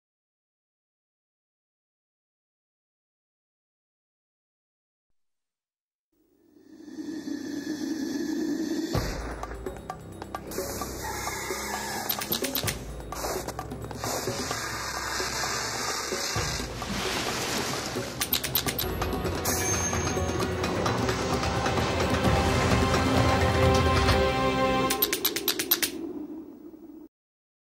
BIRDWING FILMS-PROMO
This is a small signature film for our Documentary Production Banner-Birdwing films..